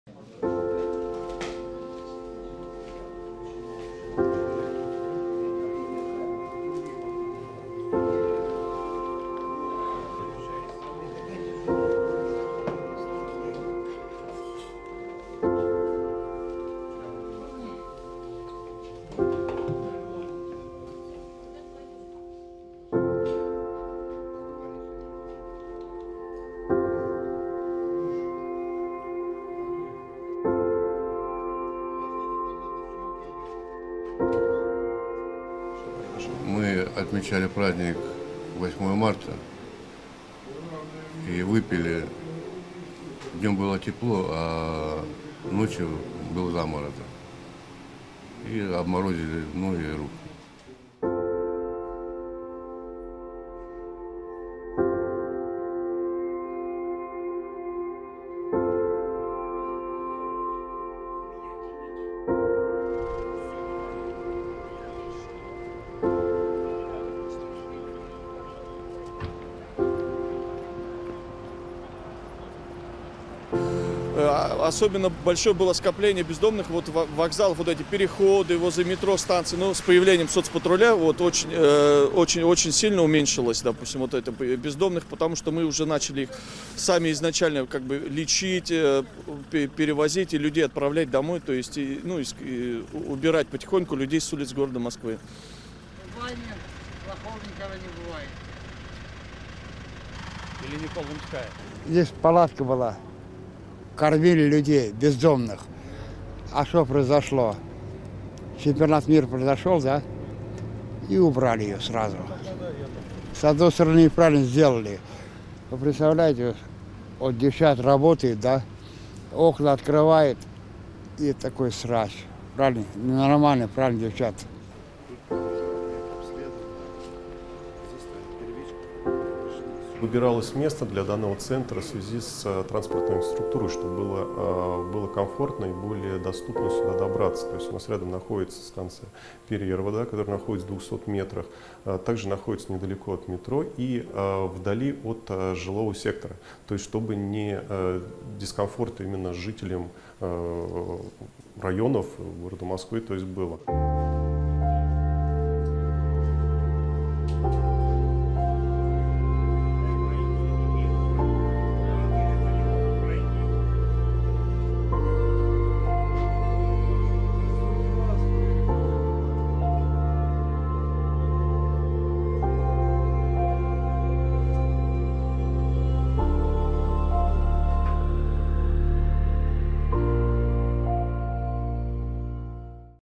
Избавиться от бомжей и спасти жизнь
Почему в Москве благотворительные учреждения для бездомных находятся вдали от жилых кварталов, и как помочь человеку, оставшемуся без крыши